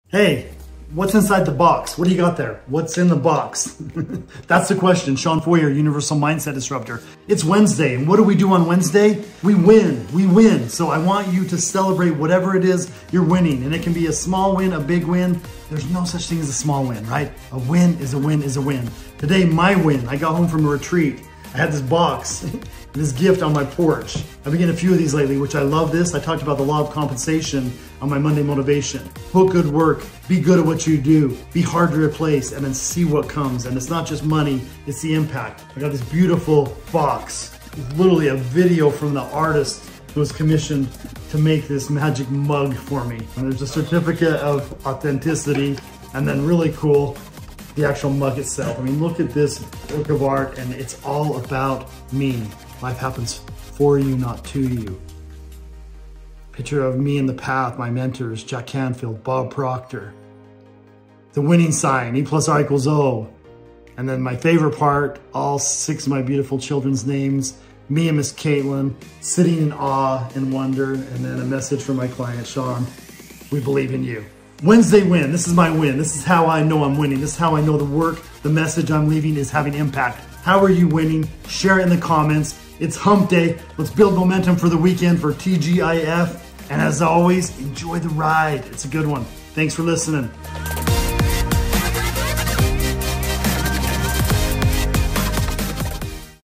This Is What A Win Looks Like
If you ever found yourself smiling over a small gift or something you absolutely love…

That's a win, right there!

It's those moments, those tiny joys, that add up to a life rich in happiness and success. 🎁

EVERY win, big or small, is another win added to our “wins collection”. 🎨

So, this Wednesday, let’s take a moment to celebrate all our wins.

Got a promotion? That’s a win. 🎉

Managed to finally organize your desk? That’s a win too. 🎉

Discovered a new song that you can’t stop playing? Yep, definitely a win. 🎉

Share your wins in the comments – big, small, or anything in between.